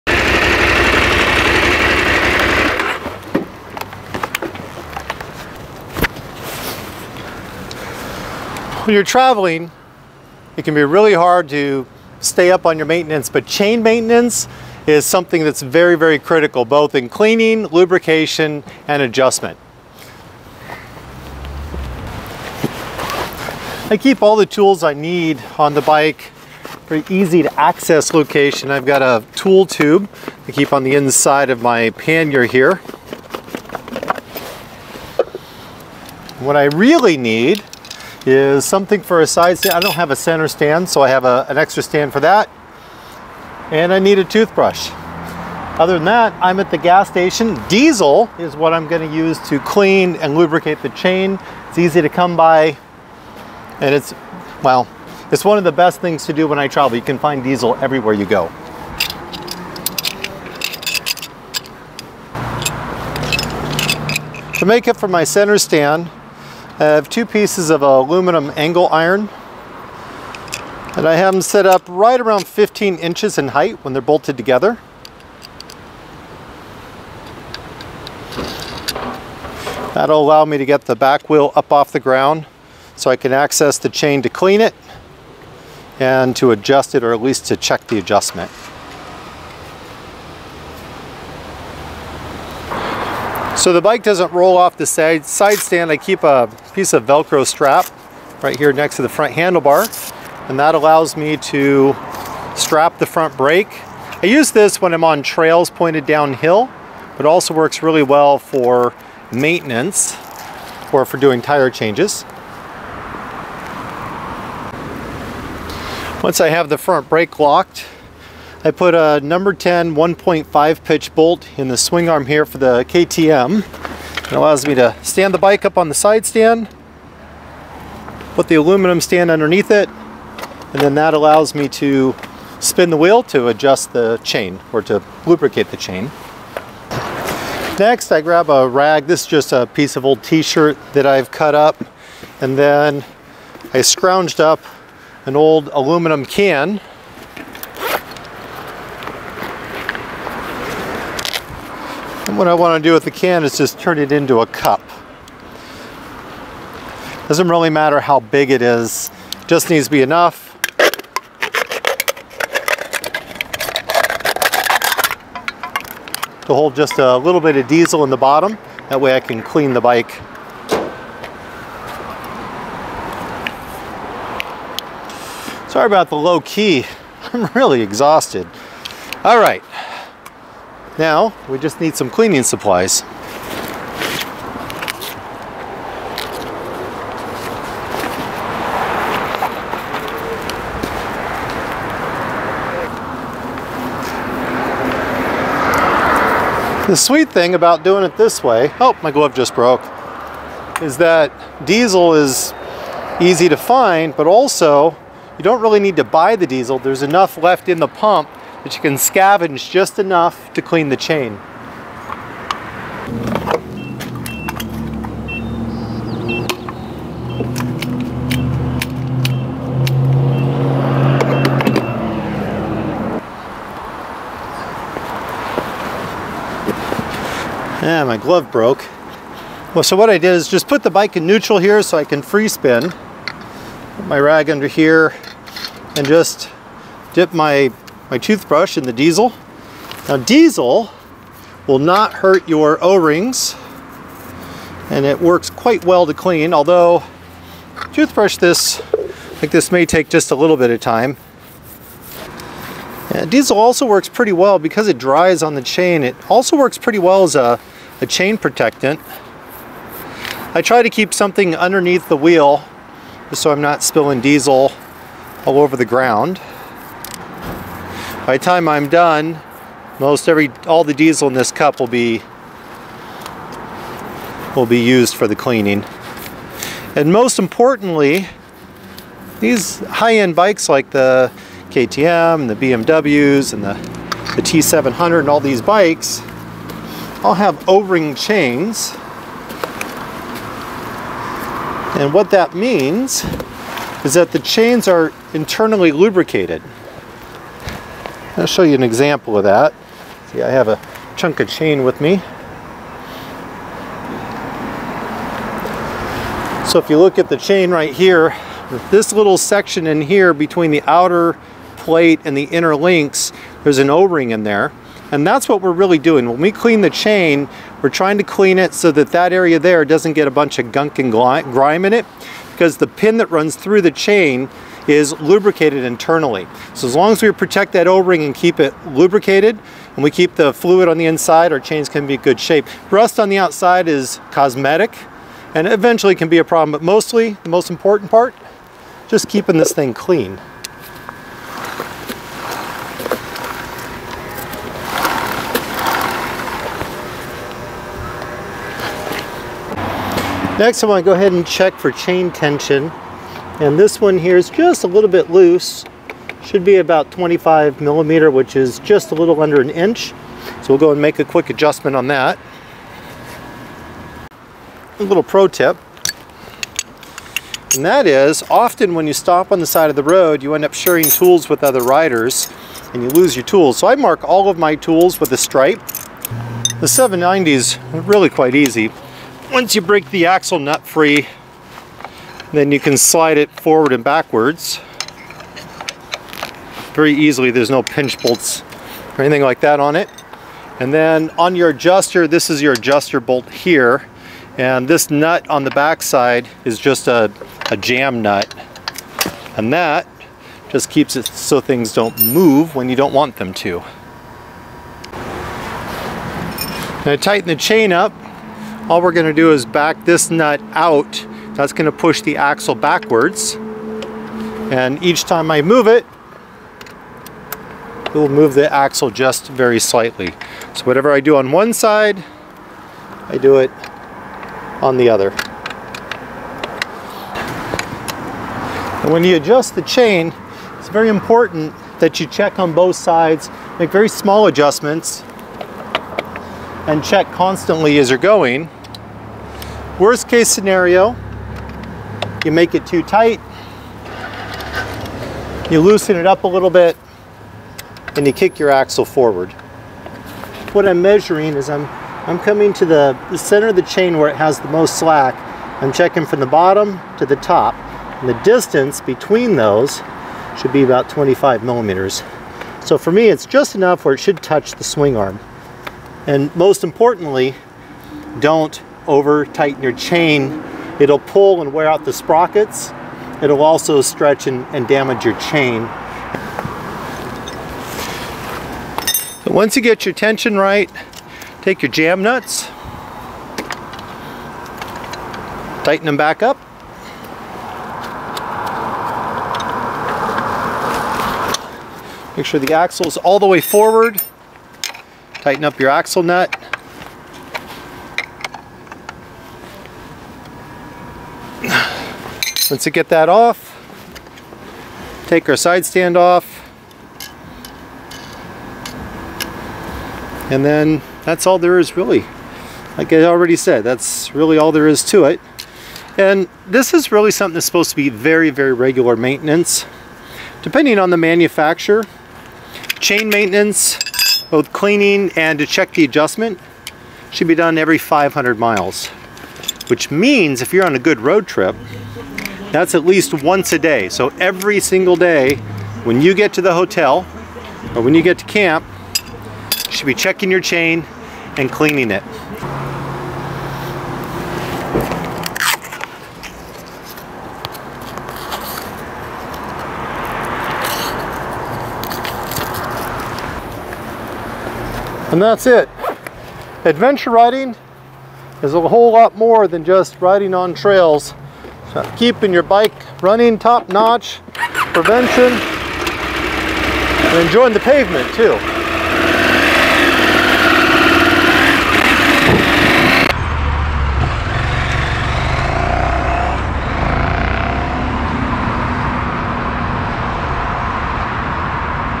Motorcycle chain maintenance tips while traveling
cleaning and adjusting your chain while traveling can be a challenge. Here are my best tips for how I clean and adjust the chain on my traveling motorcycle both local and internationally. On this trip I am riding my 2020 KTM 790 Adventure R